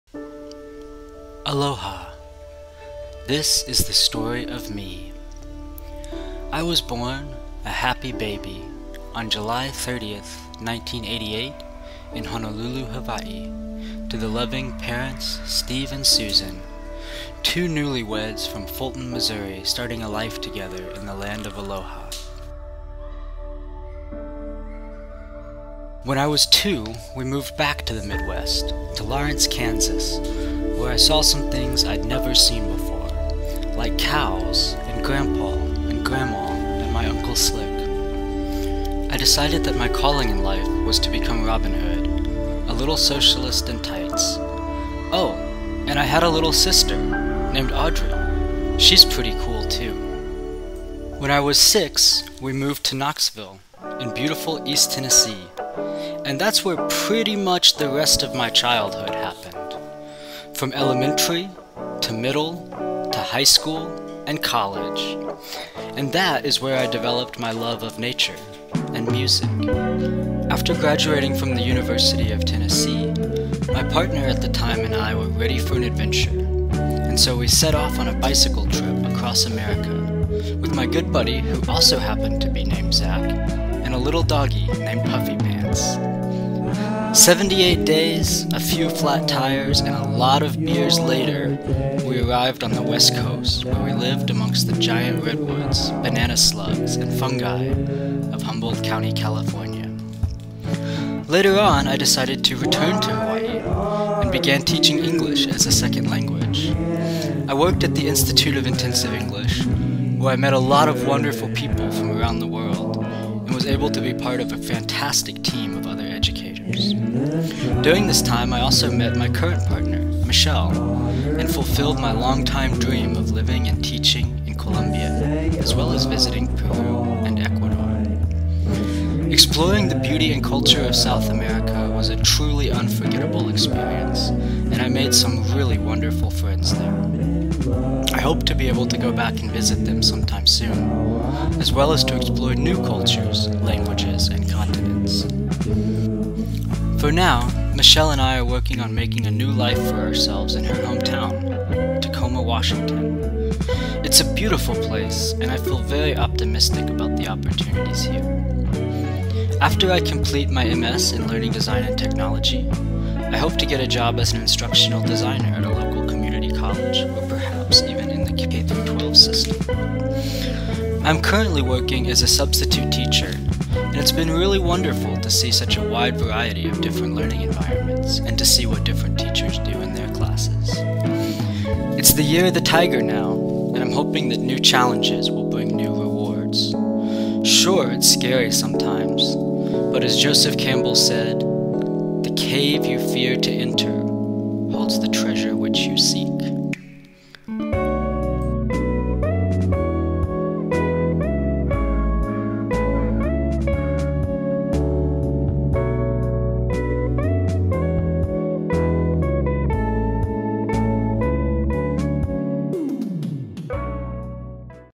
M3: The Story of Me
This video tells “The Story of ME,” from the beginning to the present day. The video was created using InShot, my own photo albums, as well as the photo albums of friends and family, and PictraMap, a tool for creating animated maps.